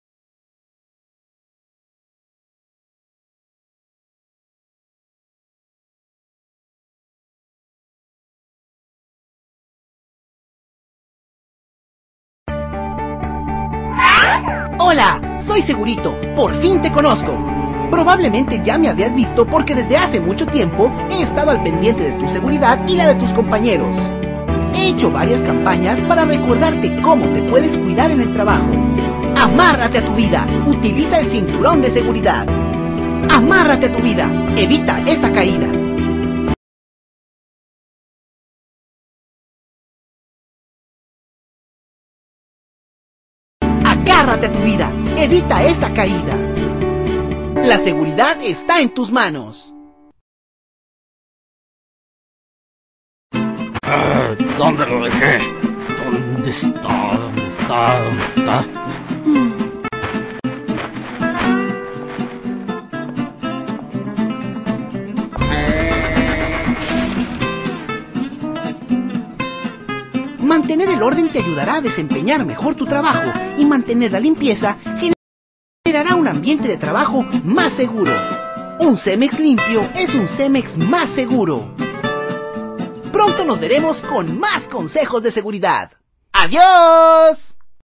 Segurito - Orden y limpieza
segurito dando consejos sobre un ambiente de trabajo ordenado y limpio